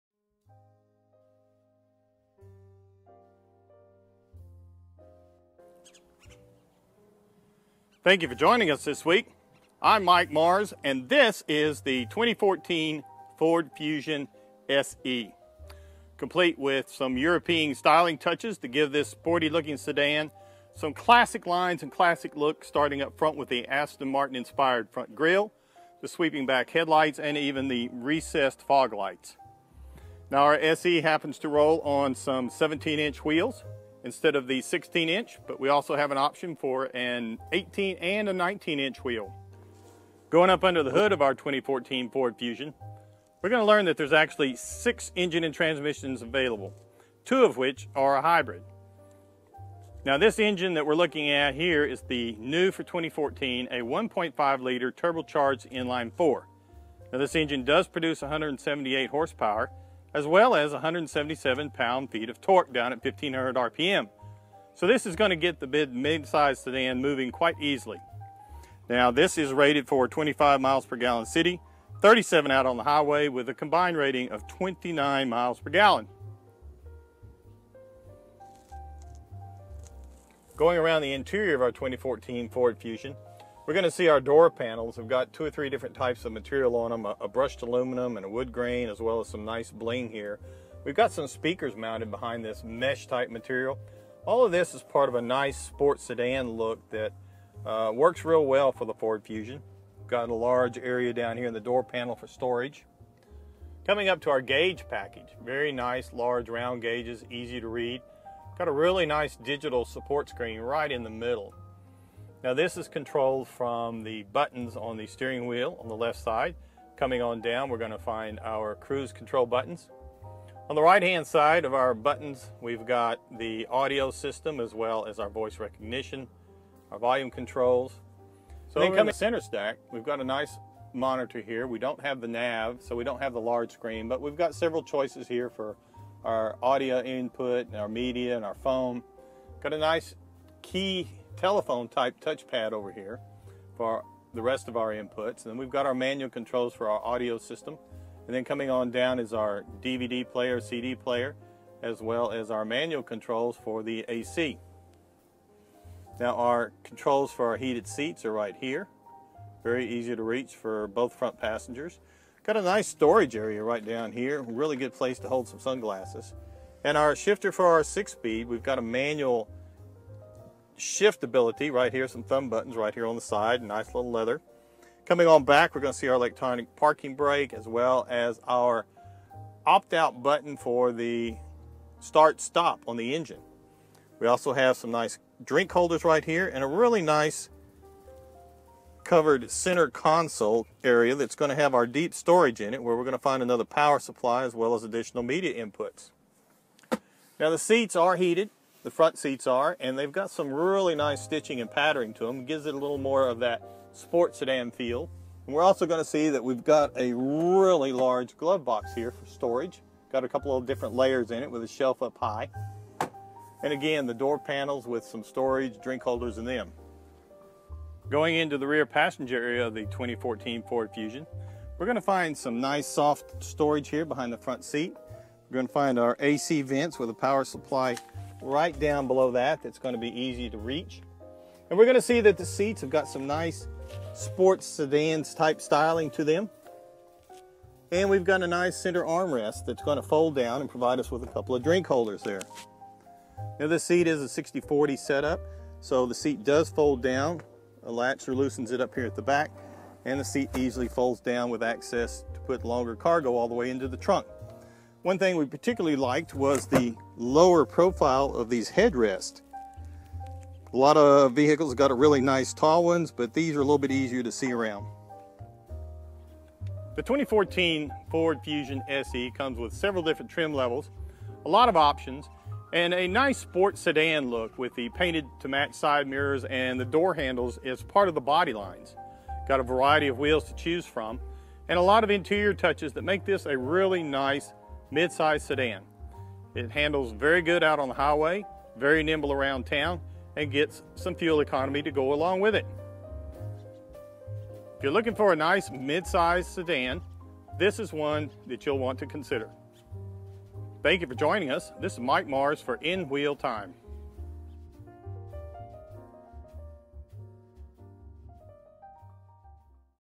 2014 Ford Fusion SE on In Wheel Time
The 2014 Ford Fusion SE is a mid-size sedan with several engine options that include turbos, automatic or manual gear box, and several trim levels that have optional feature packages.